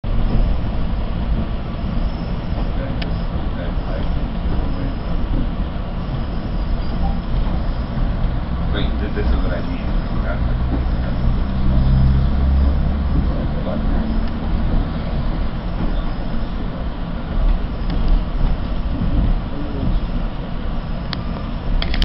Union Pearson Express Departs Union Station (September 12th 2016)
A Union-Pearson Express train, consisting of cars #3003 and #1004, departs Union Station for Pearson Airport.